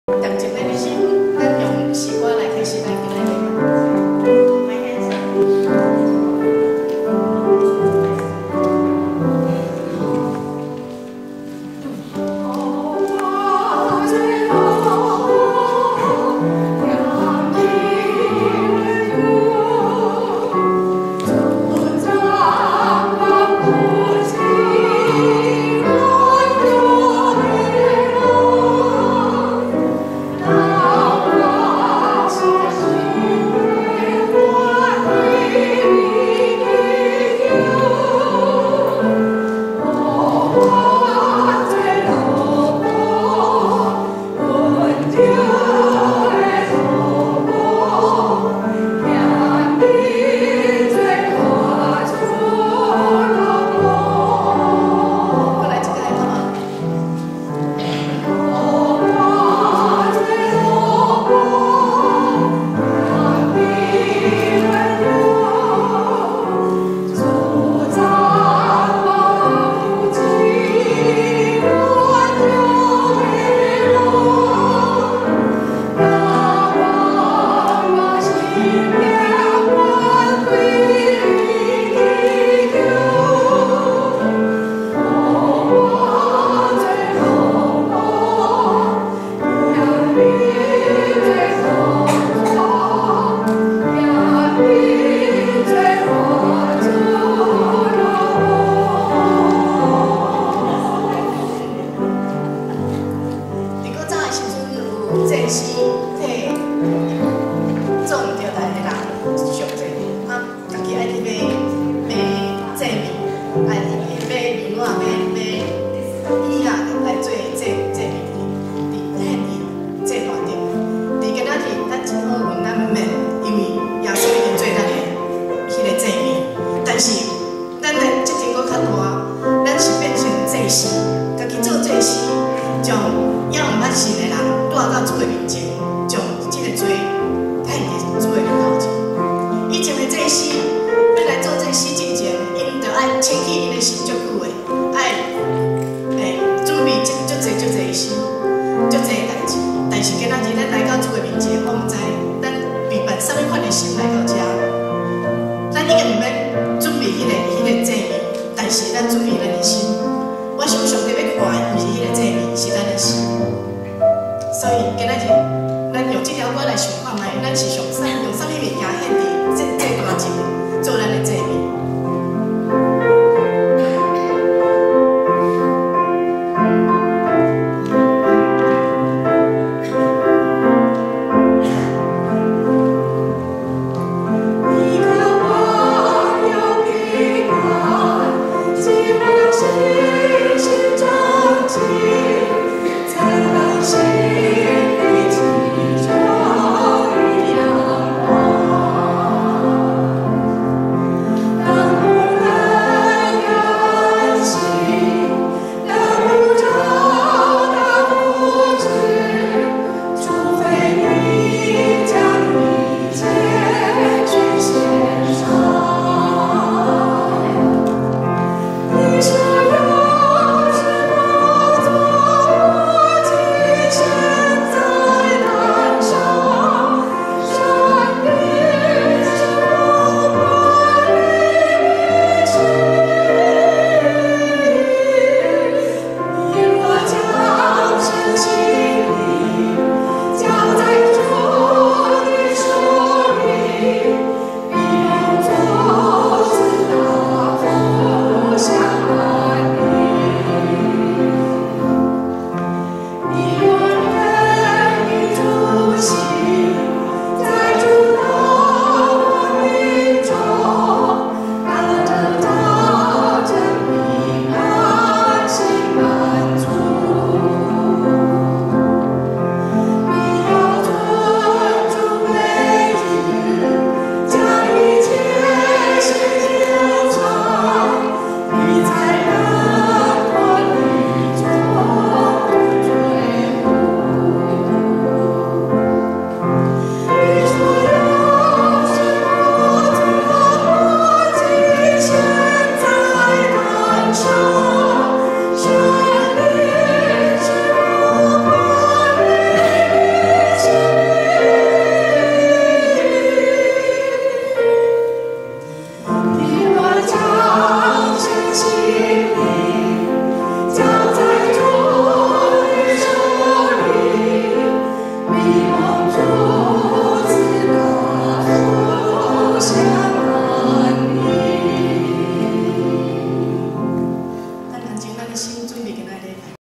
讚美敬拜領詩 - 多倫多台恩教會主日禮拜2017-05-21
多倫多台恩教會主日禮拜
領詩: 蘇芳苓、蔡艷凰、呂芳美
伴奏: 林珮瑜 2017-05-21